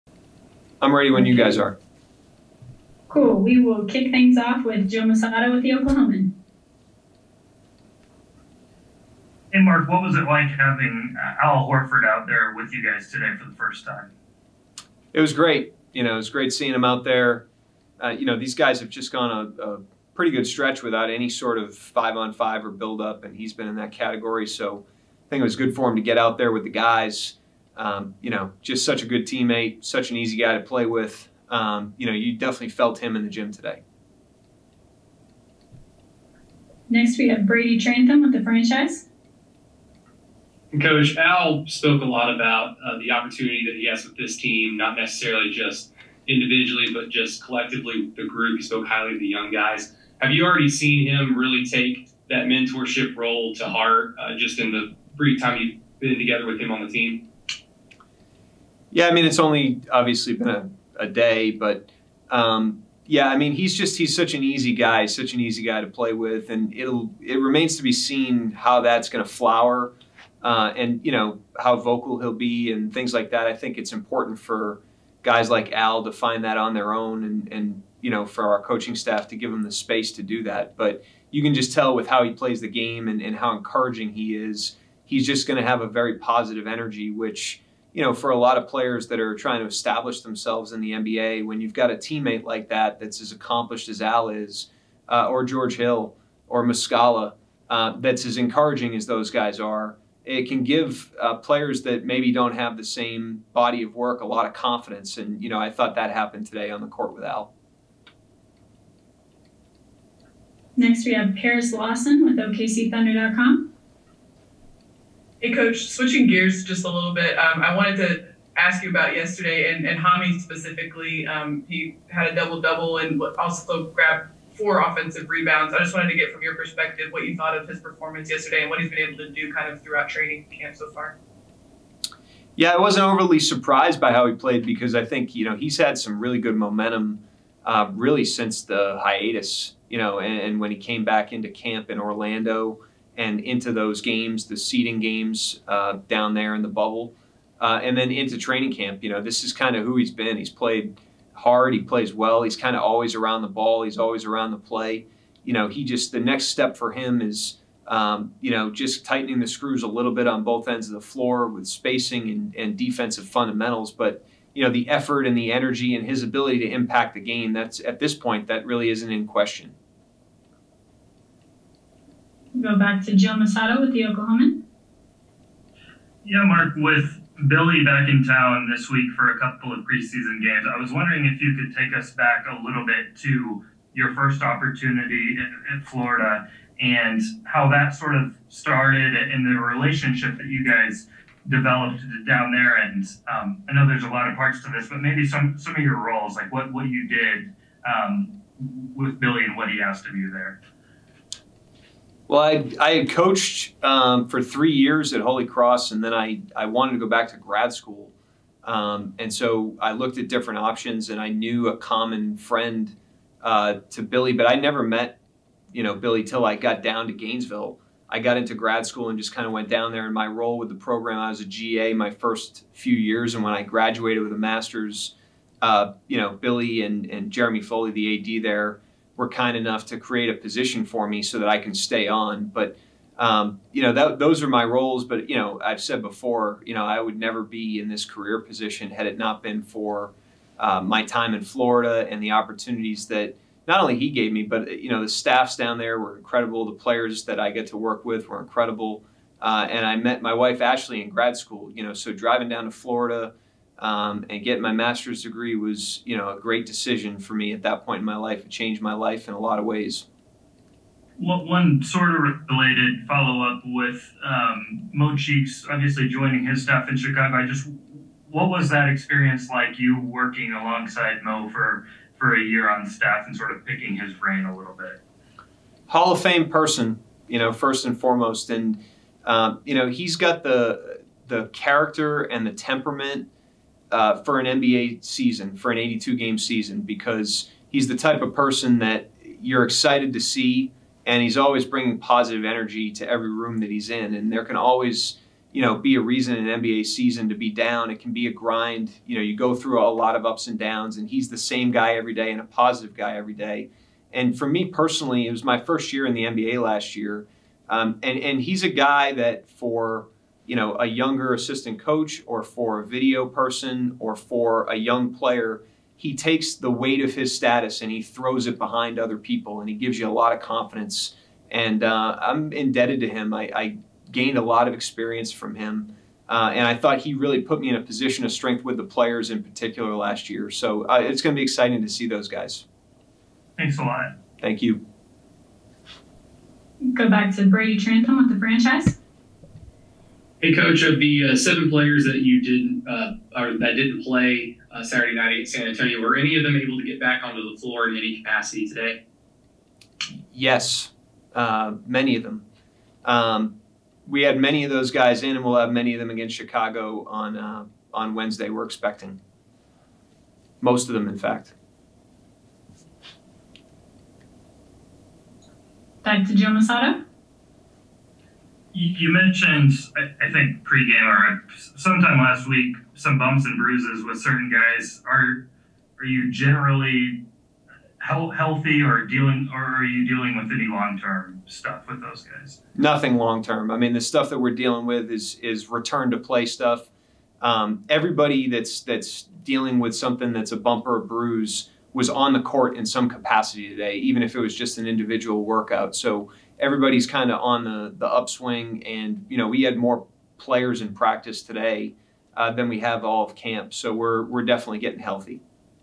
Coach Daigneault - Training Camp Media Availability 12/14/2020
Hear from Coach Daigneault after today's Training Camp practice.